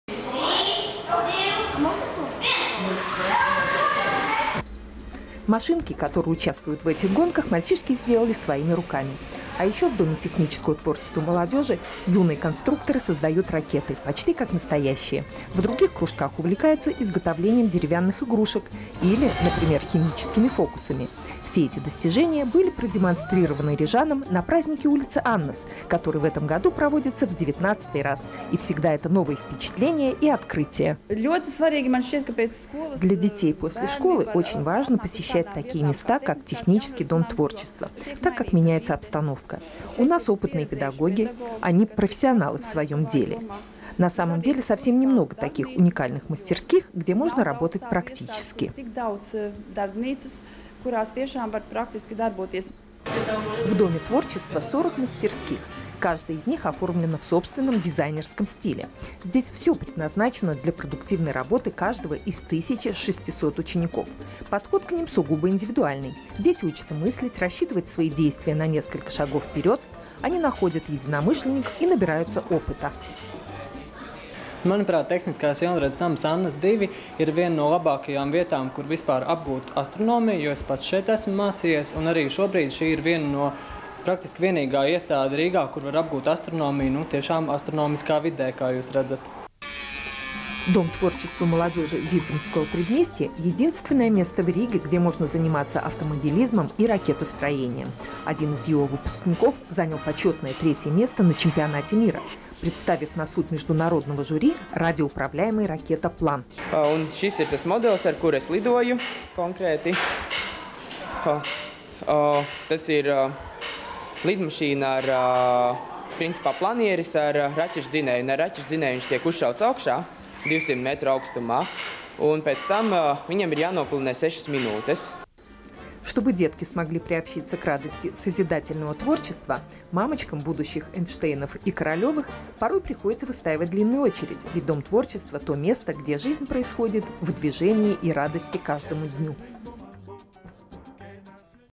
Праздник улицы Аннас
Машинки, которые участвуют в этих гонках, мальчишки сделали своими руками. А еще в доме технического творчества молодежи юные конструкторы создают  ракеты -- почти как настоящие! В других кружках увлекаются изготовлением деревянных игрушек или, напрмиер, химическими фокусами.  Все эти достижения были продемонстрированы рижанам на   традиционном «Празднике улицы Аннас», который в нынешнем году проводится уже в 19-й раз. И всегда это -  новые впечатления и открытия.
Ирена Маскалёнока, директор:  Я считаю, что для детей, после школы очень важно посещать такие места как технический дом творчества, так как меняется обстановка. У нас опытные педагоги, которые профессионалы в своём деле. На самом деле совсем не много таких уникальных мастерских, где можно работать практически.
В доме творчества 40 мастерских, каждая из них оформлена в собственном дизайнерском стиле. Здесь все предназначено для продуктивной работы каждого из тысячи шестисот учеников. Подход к ним -- сугубо индивидуальный. Дети учатся мыслить, рассчитывать свои действия на несколько шагов вперёд; они находят единомышленников и набираются опыта.
Дом творчества молодежи Видземского предместья - единственное место в Риге, где можно заниматься кордовым автомобилизмом и ракетостроением. Один из его выпускников занял почетное третье место на чемпионате мира!, представив на суд международного жюри  радиоуправляемый ракетоплан.
Лаурис Пумпурс: Это модель, с которой я участвую в соревнованиях. Это планэр с ракетным двигателем. Он взлетает вверх на двести метров и ему необходимо продержаться шесть минут.
Чтобы детки смогли приобщиться к радости созидательного творчества,  мамочкам будущих Эйнштейнов и Королевых порой приходится выстаивать длинную очередь.  Ведь дом творчества - то место, где жизнь происходит в движении и радости каждому дню.
Дом технического творчества Annas2
Рига, ул.Аннас, 2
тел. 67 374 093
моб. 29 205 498